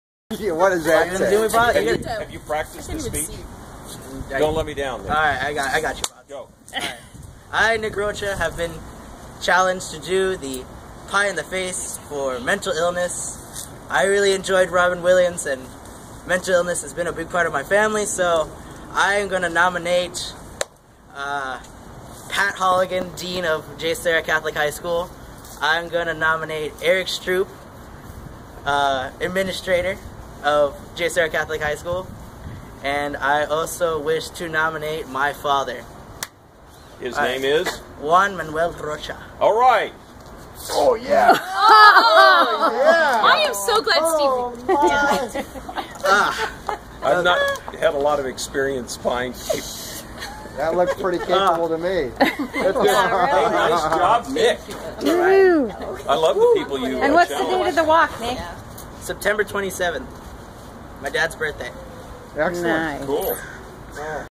Nicolas Rocha Pie in the Face Challenge Video (NAMI OC)
Nicolas accepting the Pie in the Face Challenge.